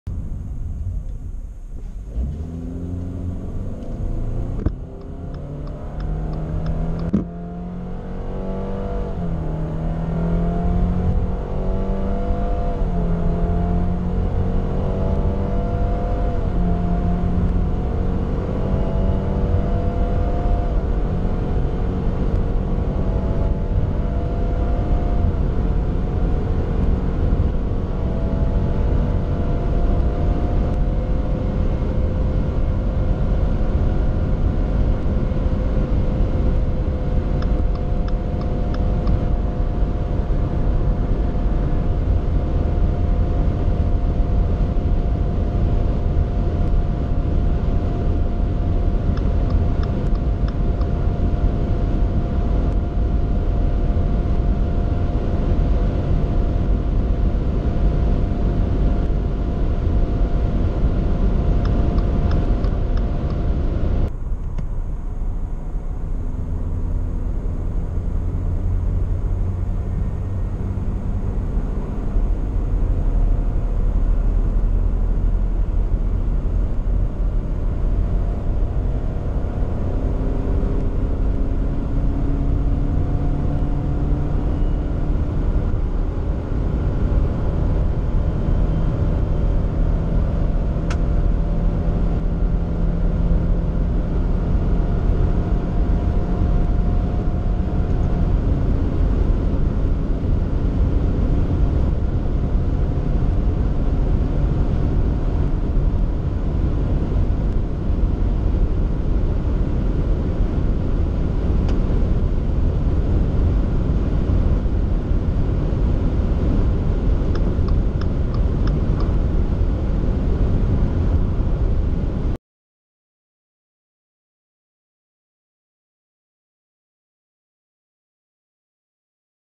2018 Subaru XV 2.0i Lineartronic: Beschleunigung 0 - 200 km/h - Autophorie
Beschleunigung des 2018 Subaru XV 2.0i mit dem 156 PS starken Boxermotor.

Technische Daten

1.995 ccm Hubraum; 2,0-Liter Boxermotor
115 kW / 156 PS @ 6.000 U/min
196 Nm @ 4.000 U/min
10,4 Sek auf 100 km/h
194 km/h V-Max
6,9 l/100km
155 Gramm CO2 pro km
Abgasnorm Euro 6

1.400kg Anhängelast; 1.462kg Leergewicht
4.465mm lang; 1.800mm breit; 1.615mm hoch; 2.665mm Radstand

Bodenfreiheit 22 Zentimeter! Böschungswinkel v/h: 19,7° / 31,5°
Rampenwinkel: 21,9°

Versicherung: KH 17 / VK 21 / TK 22
Service: Ölwechsel 12 Monate oder 15.000km; Inspektion 24 Monate oder 30.000km